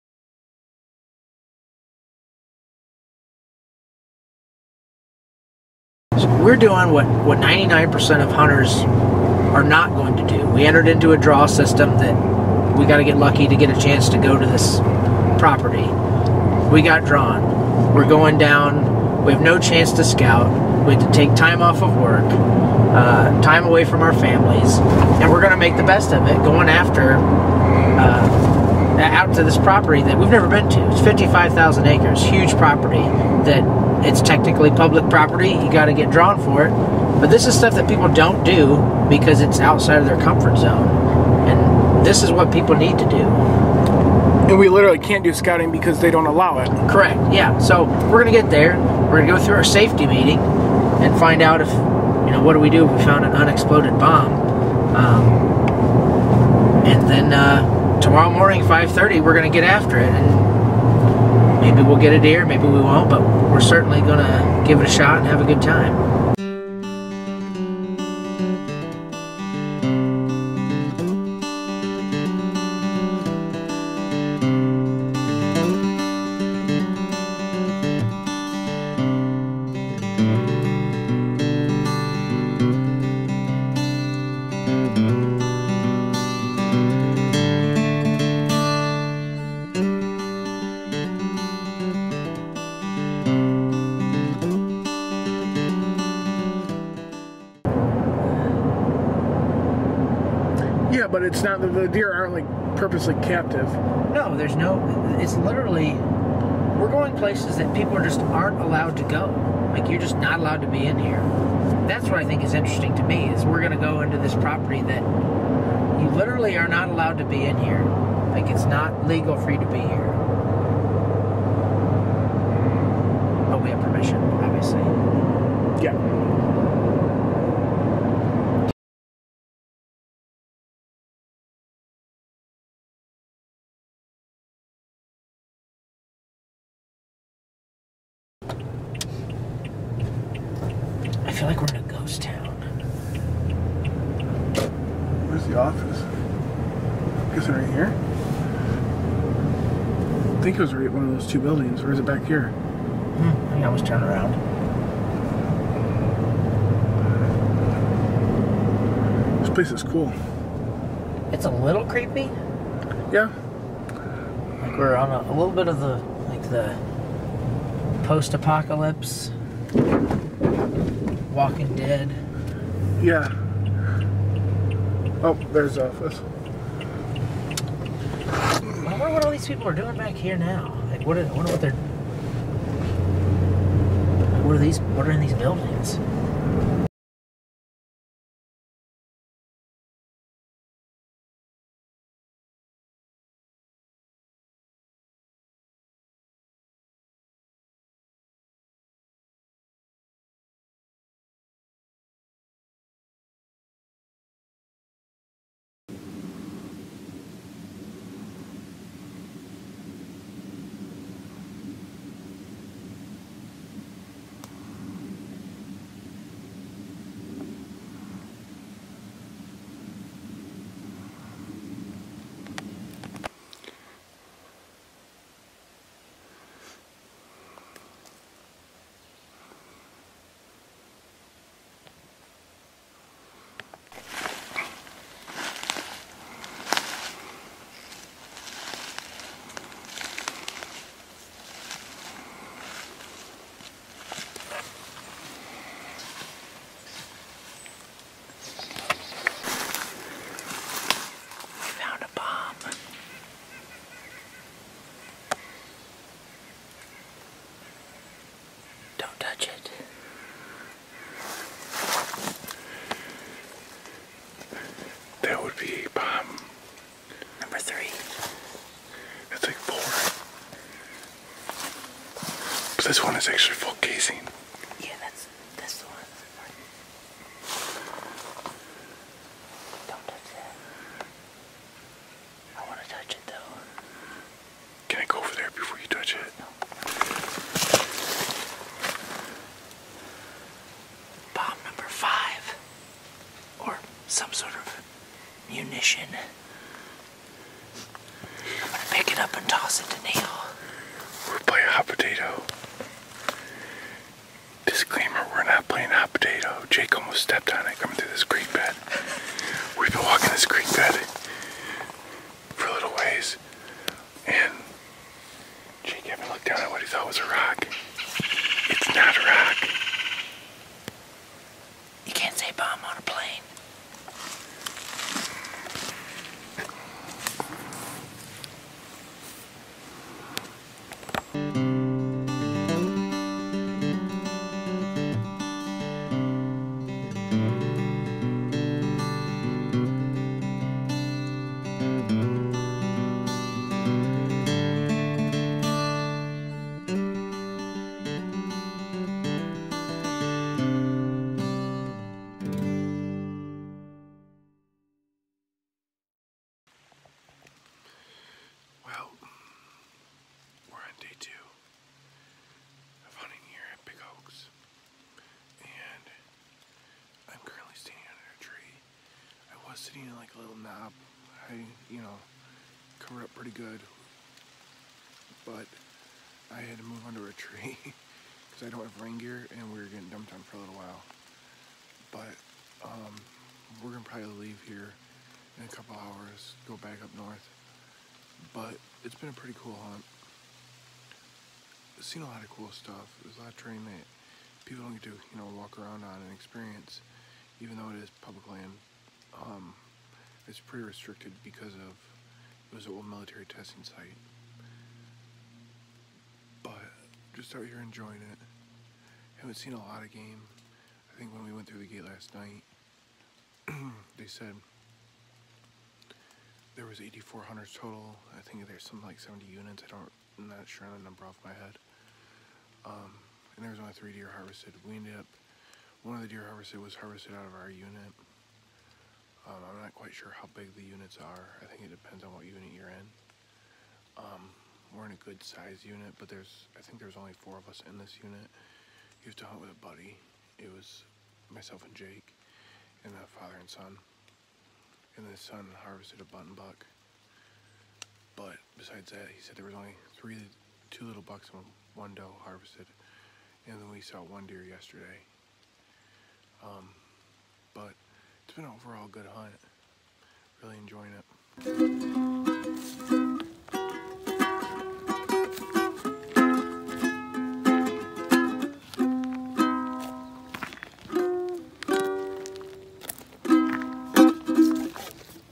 Hunting a Military Testing Base
My buddy Jake drew a special reserve hunt tag in Indiana to hunt Big Oaks Wildlife Refuge, which parts of it still sees some military testing. Throughout this 55,000 acre piece of land, it is littered with some old munition shells that we had to be aware of trudging through this beautifully weird piece of property. ( We did not touch nor contact any old munition rounds).

We had a great time on this piece of property even though our deer sitings were limited. A couple factors went into that, one was that we were unable to scout the prior days due to park regulations. All in all, it was a fun hunt, and a great adventure.